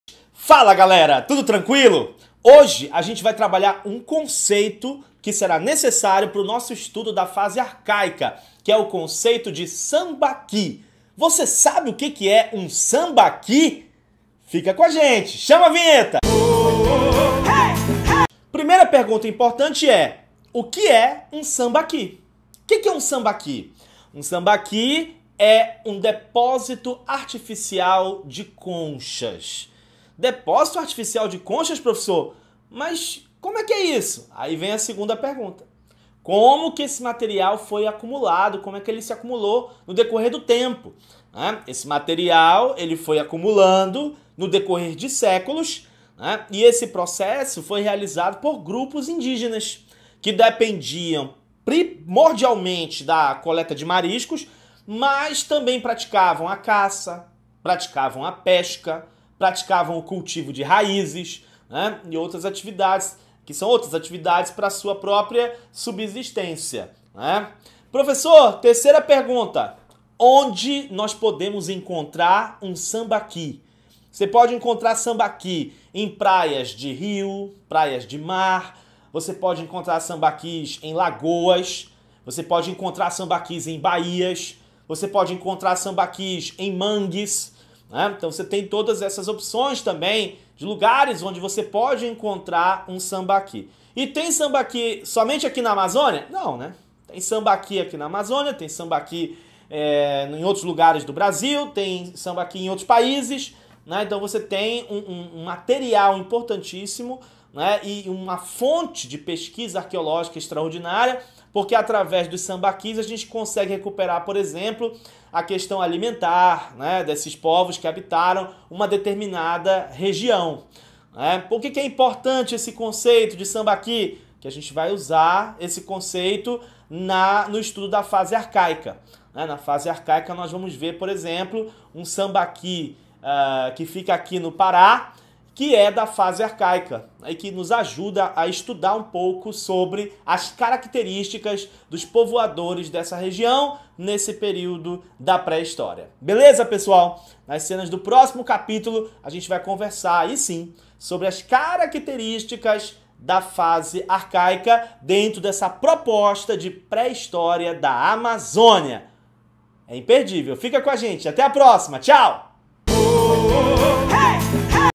O QUE É UM SAMBAQUI?
Curso completo:
Os sambaquis são importantes para a pesquisa arqueológica. Aqui trabalhamos o conceito e suas características.

História e outras Humanas para vestibulares e concursos você encontra aqui no canal Senhor da História! Seja bem-vindo!